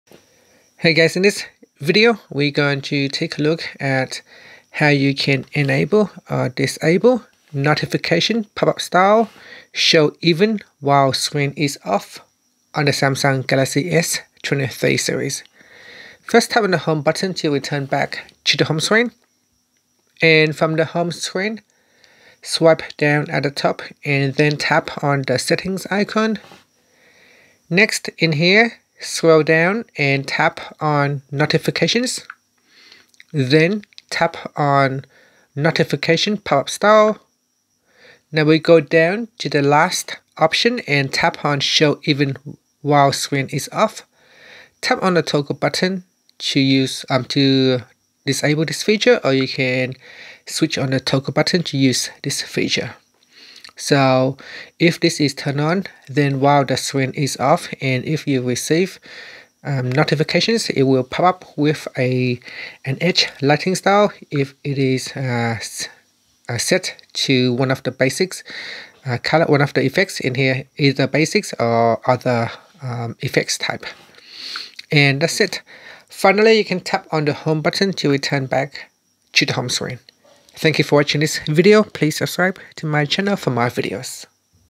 Galaxy S23's: How to Enable/Disable Notification Pop-up Style Show Even While Screen Is Off
Learn how you can enable or disable Notification Pop-up Style Show Even While Screen Is Off on the Samsung Galaxy S23/S23+/Ultra.

The purpose of turning on/off edge lighting notifications while the screen is off on a Samsung Galaxy S23 is to provide a visual cue for incoming notifications, calls, and other events even when the screen is not active or unlocked.

When edge lighting is turned on, the sides of the screen will light up with a customizable color when you receive a notification or call. This can be especially useful in situations where you don't want to turn on your phone screen or unlock your device, but you still want to know if you have received a notification or call.